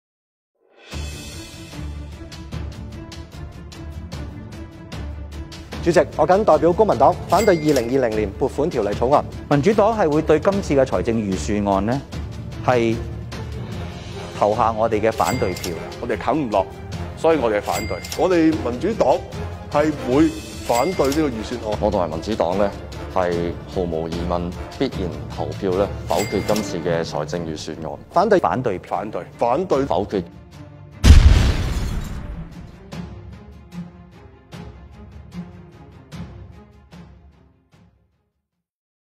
【認住呢班攬炒派 唔俾市民有一萬】
攬炒派喺唔同場合都表示過會反對預算案，攬炒全民一萬蚊，而其中呢幾個仲喺會議上表明會否決財案添，果然身體最誠實！當日屈人反對派錢，自己原來先係真正「反對派」，一齊睇下佢哋有幾「誠實」啦。

🙏🙏請大家支持：
2. 撳🔔搶先睇
3. 讚好並留言支持
4. 將影片分享開去🙏🙏